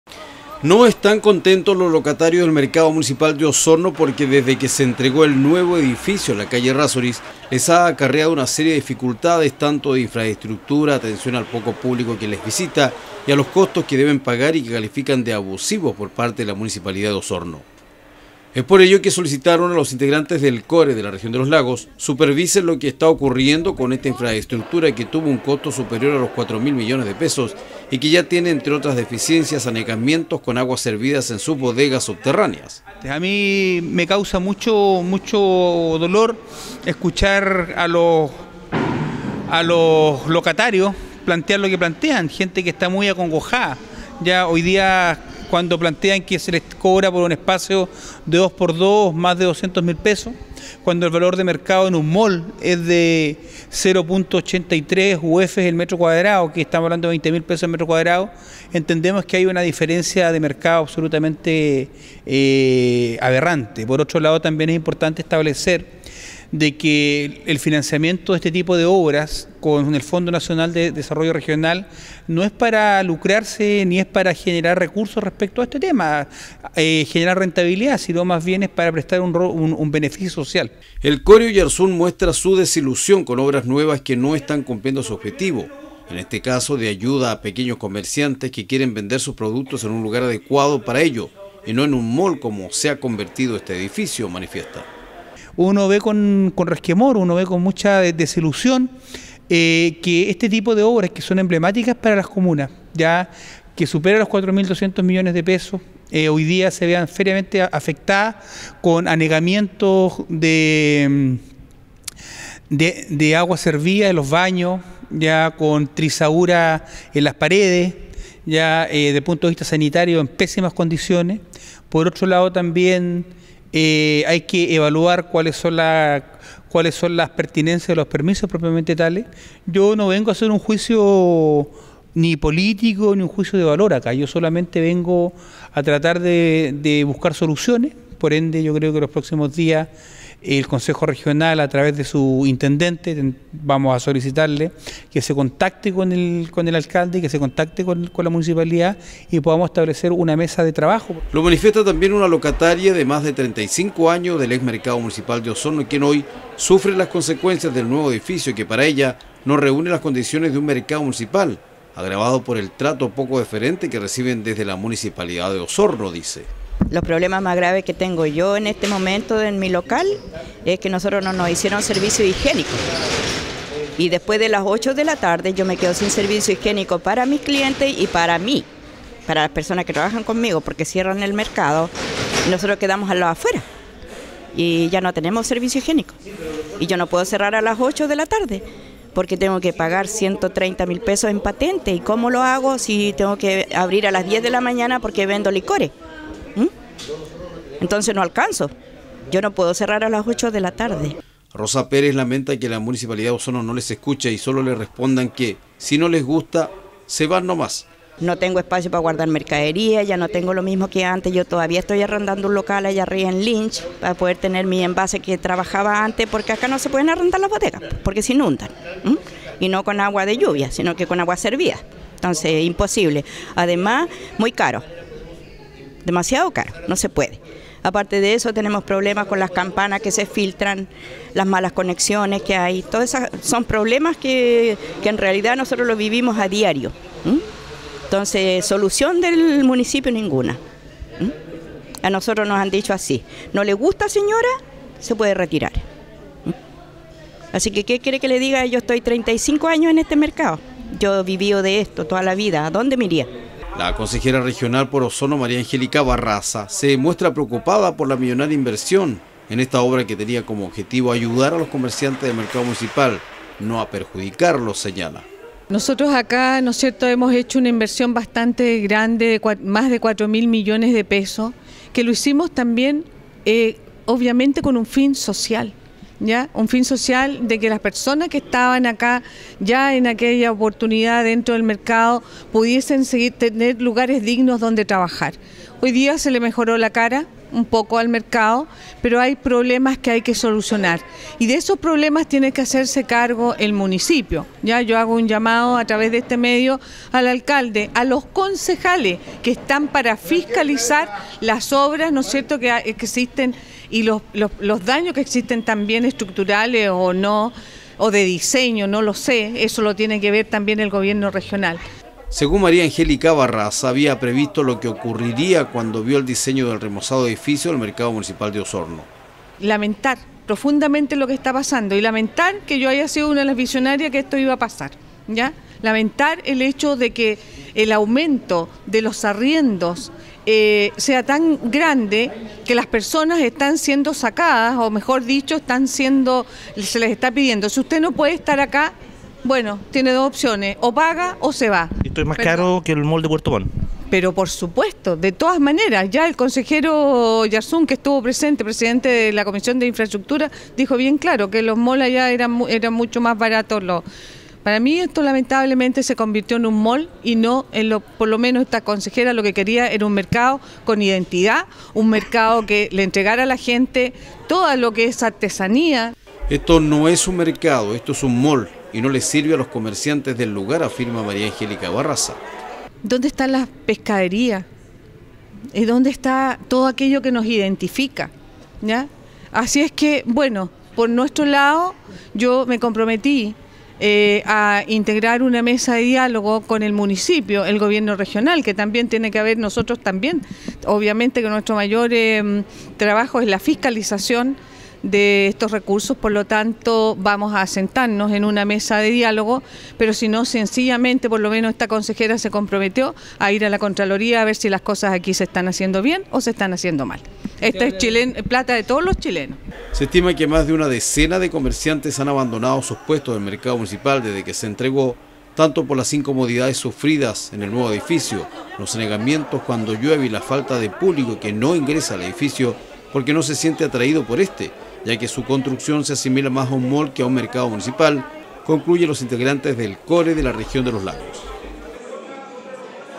Deficiencias de infraestructura y costos enfrentan locatarios del Mercado Municipal
Integrantes del Consejo Regional de Los Lagos supervisaron el trabajo de locatarios del nuevo Mercado Municipal de Osorno, detectando una serie de deficiencias, tanto de infraestructura como de cobros excesivos por local a comerciantes que venden poco y nada de sus productos.